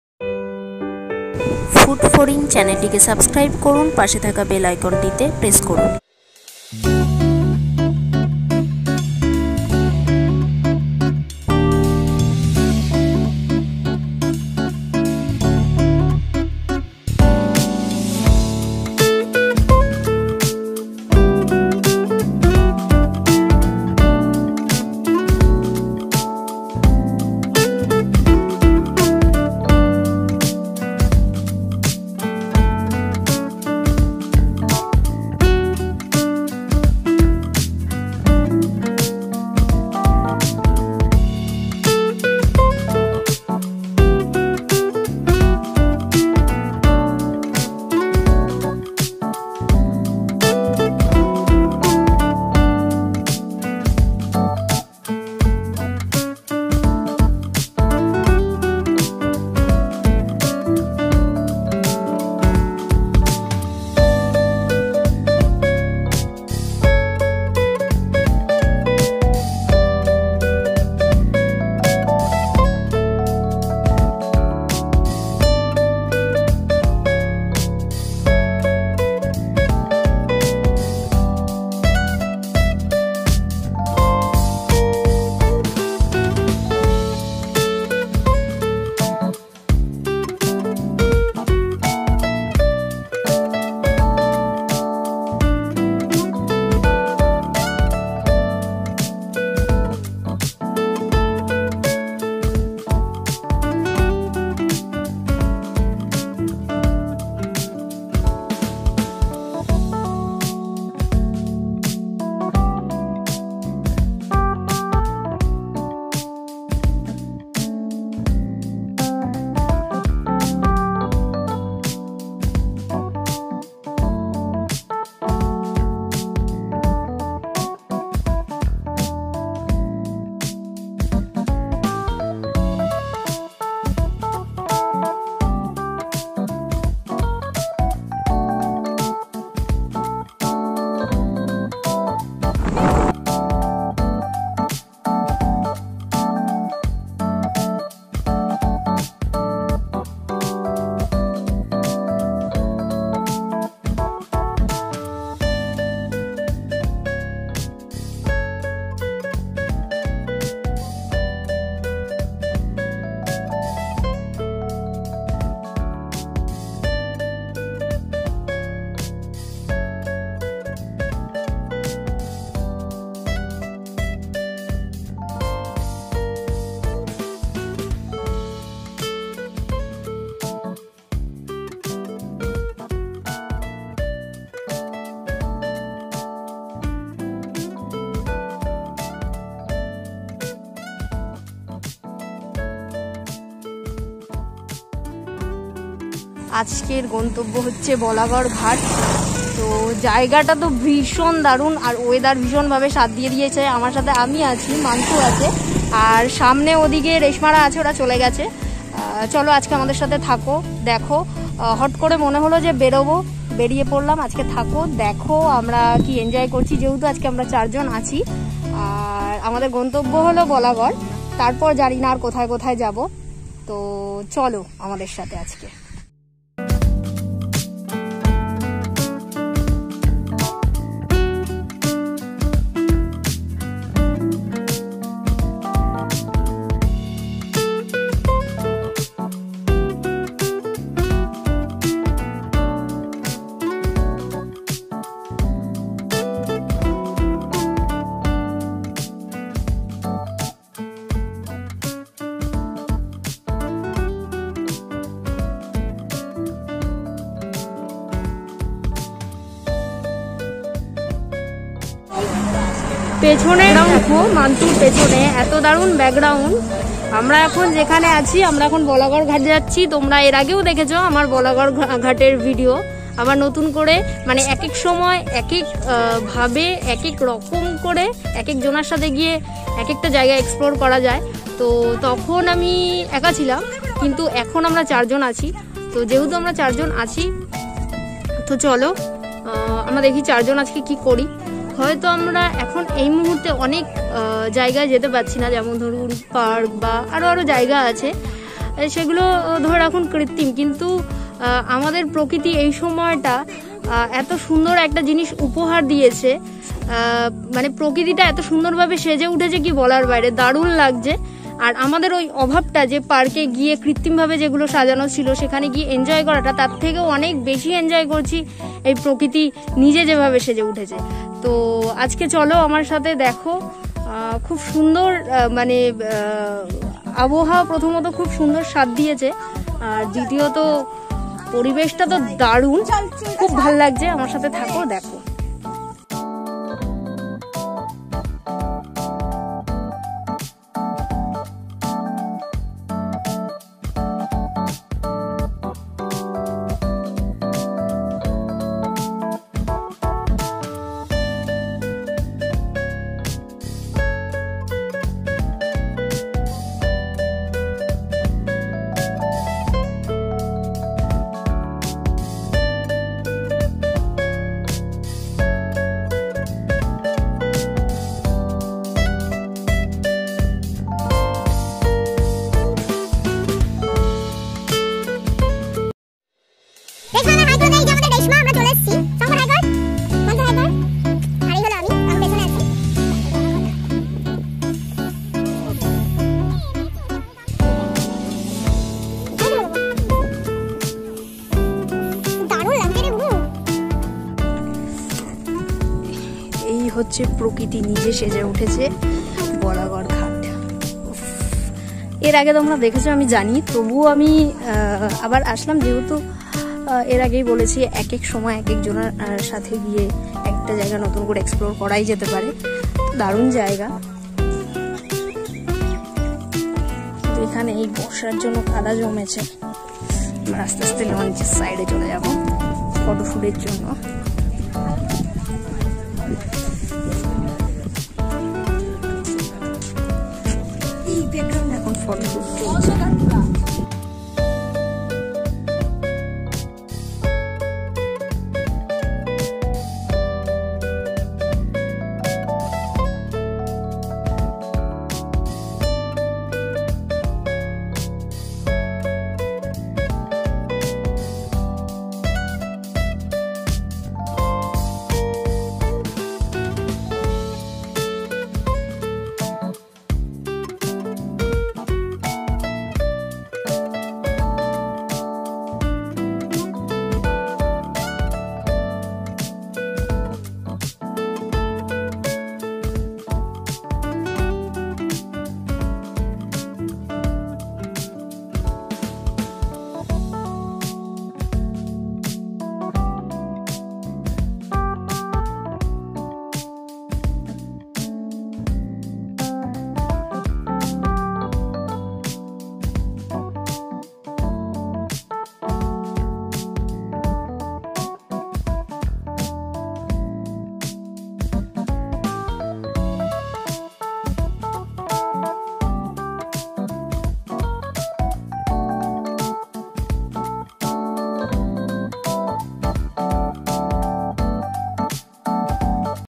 প্রকৃতির টানে ছুটলাম বলাগরঘাট
নদীয়ার একদম কাছে নিজের মতো করে সময় কাটানোর জায়গা সাথে প্রকৃতি ছোয়া